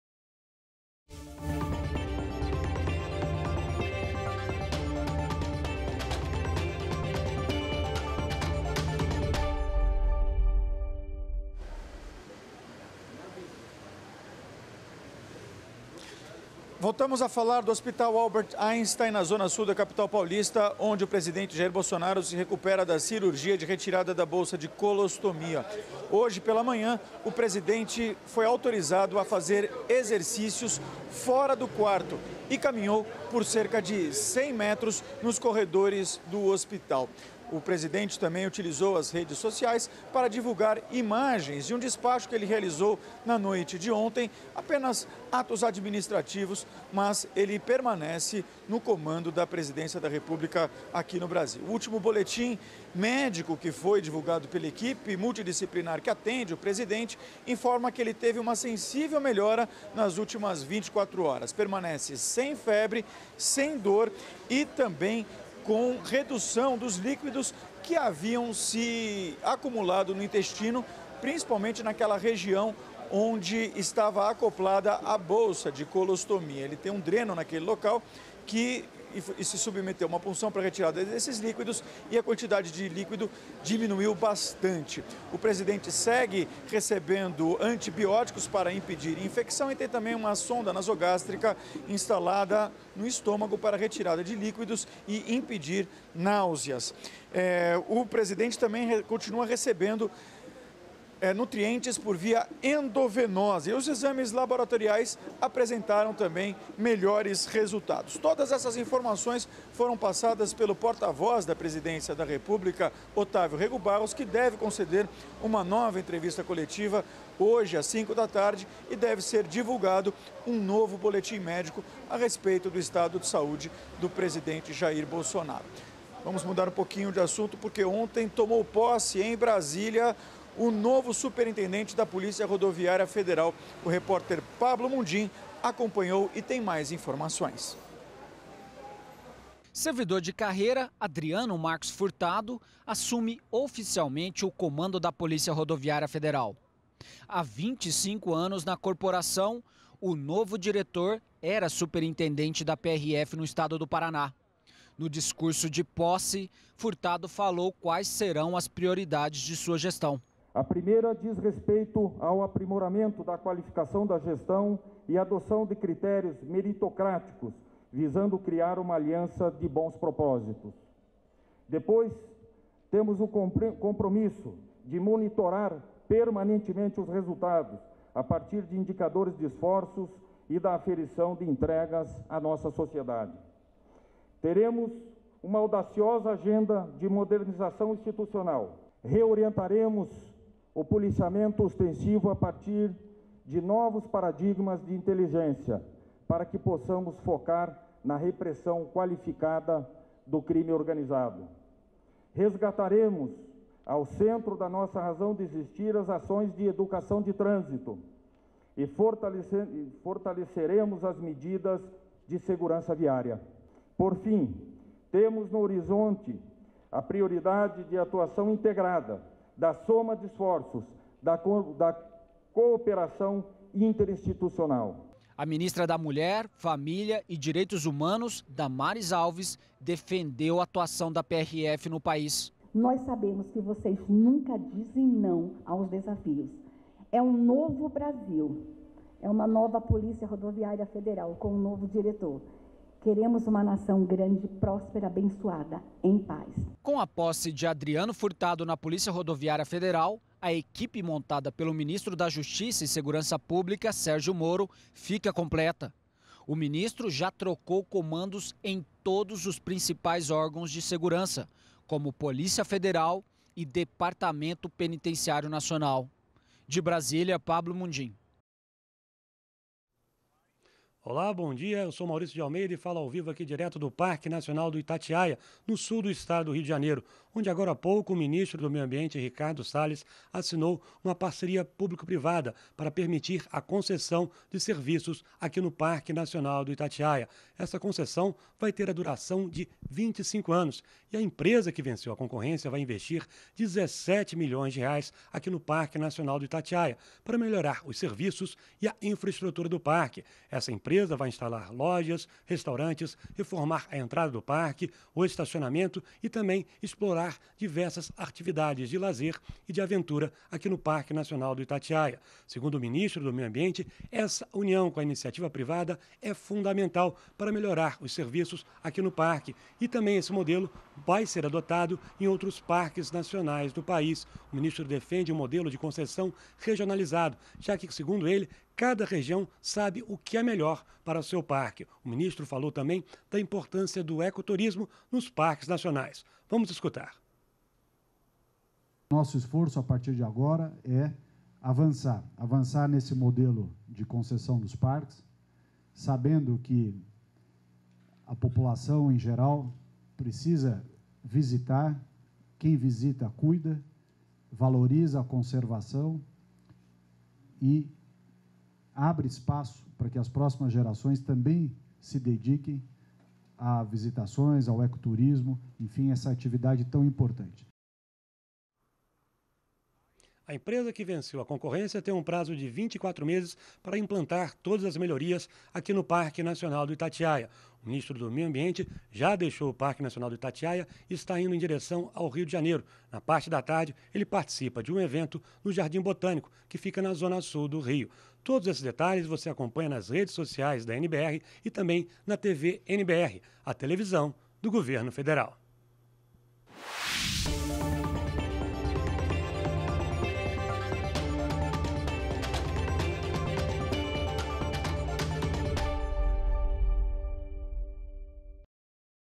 Adriano Marcos Furtado assume direção da Polícia Rodoviária Federal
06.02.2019 - Presidente Jair Bolsonaro continua se recuperando bem da cirurgia para retirada da bolsa de colostomia. Ex-Superintendente da Polícia Rodoviária Federal do Paraná é o novo Diretor Geral da PRF. Ministério do Meio Ambiente fecha Parceria Público Privada para concessão de serviços no Parque Nacional do Itatiaia, no Rio de Janeiro.